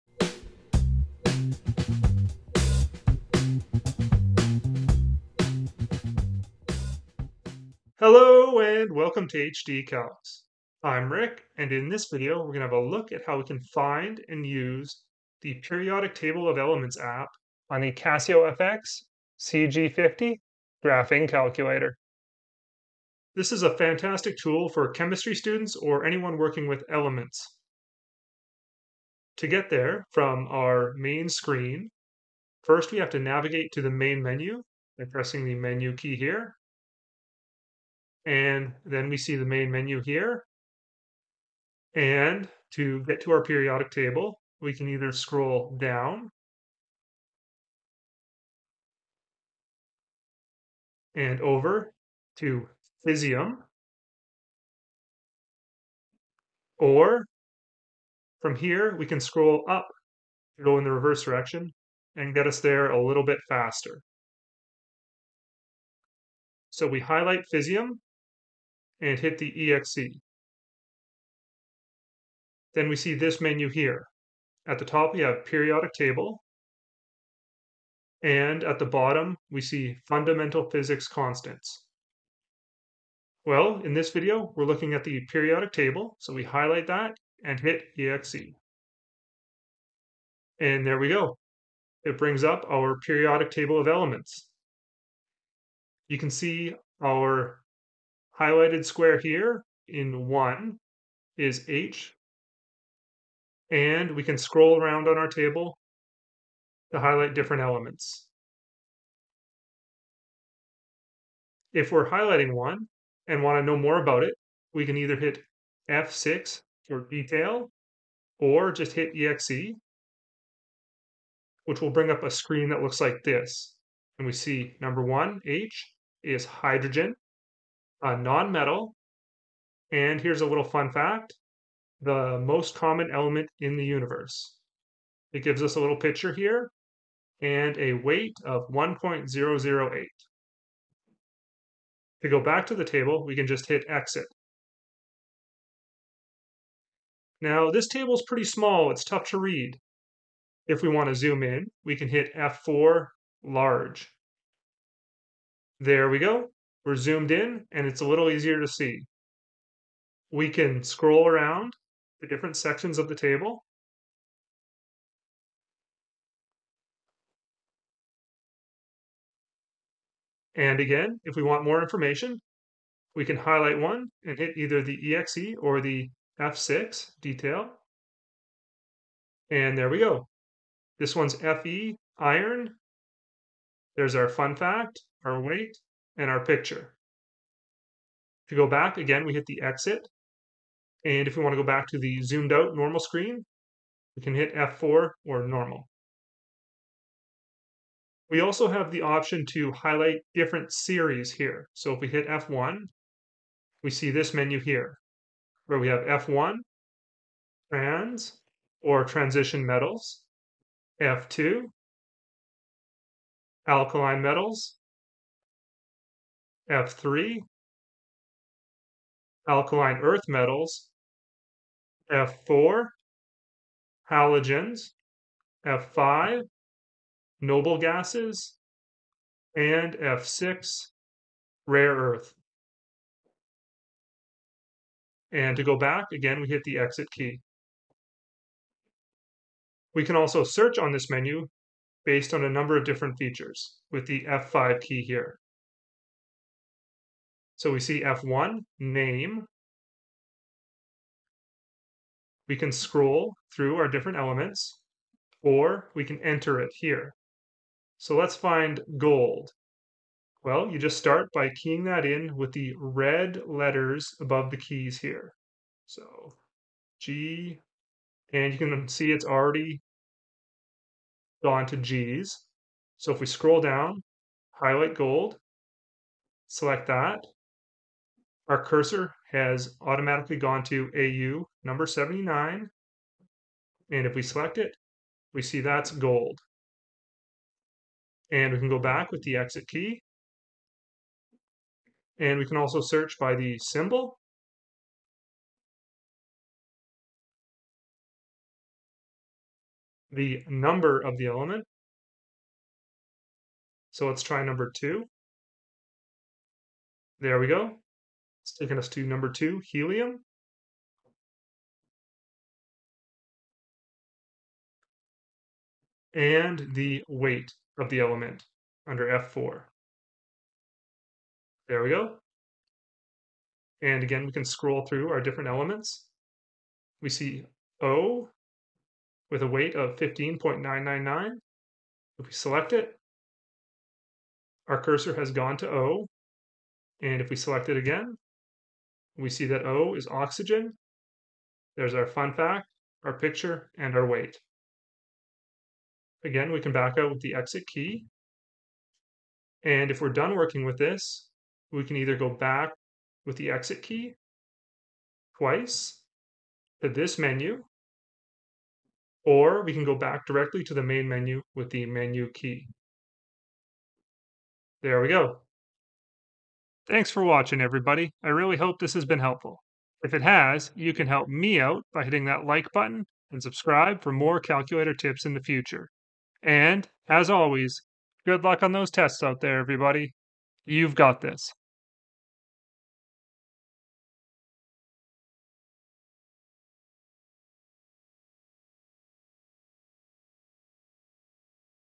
How to use the Periodic Table of Elements on the Casio FX-CG 50
A quick look at how to find, search and use the periodic table of elements on the Casio fx-cg50 graphing calculator. You can use this app to search elements based on their name, symbol and weight, look up fun facts and use them in calculations.

Contents:
0:00 Intro
0:28 Accessing the App
1:50 Selecting an Element
2:26 Zooming and scrolling on the table
3:17 Select an Element Series
3:59 Searching by name
4:51 Searching By Number
4:59 Searching by Atomic Number
5:15 Searching by Atomic Weight
5:49 Return to Main Menu
6:06 Outro